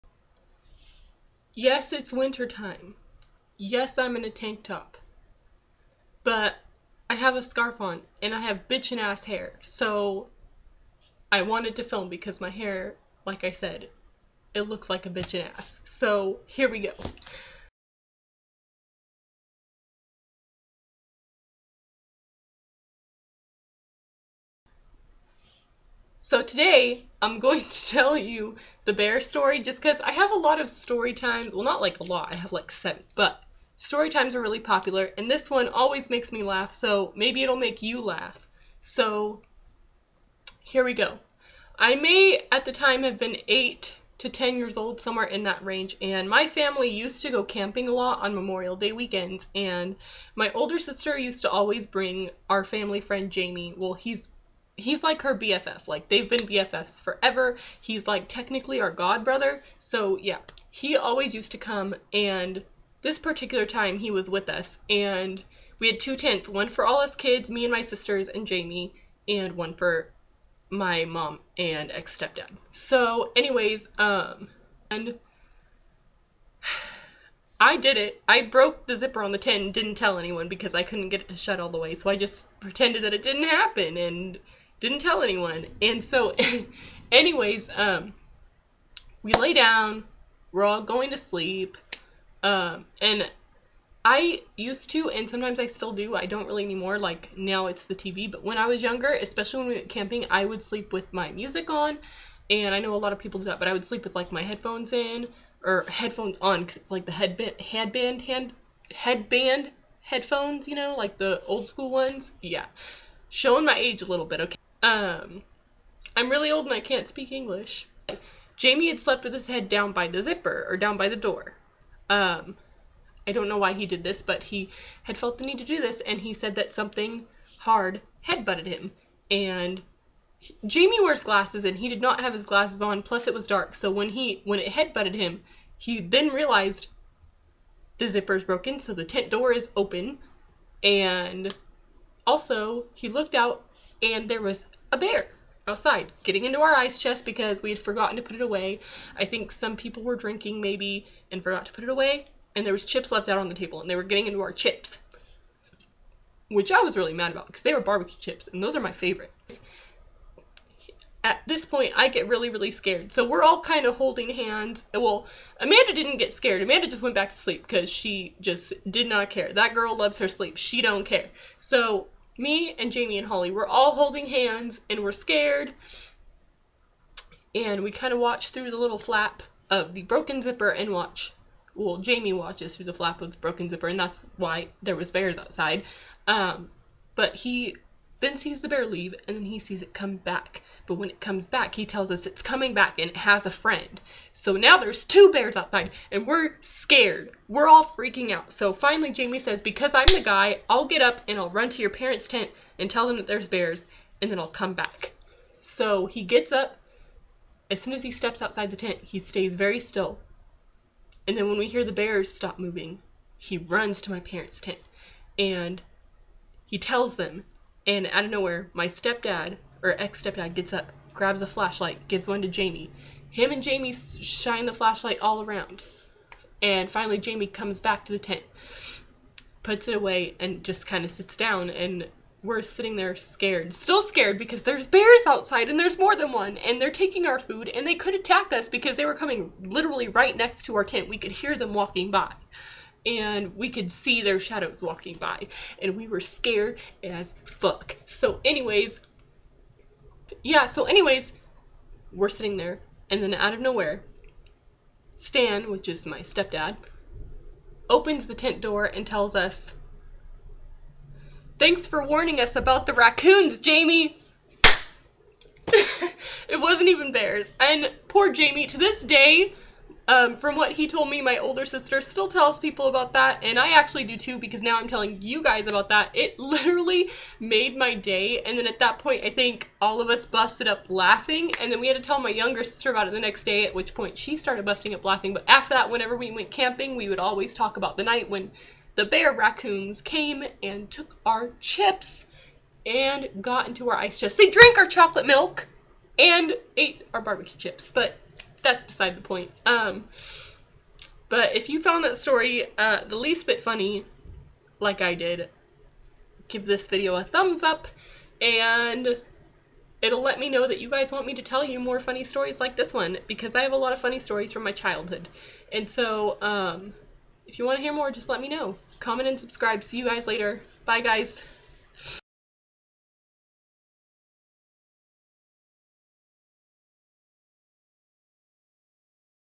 Story Time Theater: Bear Attack
Hey guys! Here's a funny childhood story for you on how my family was almost attacked by bears!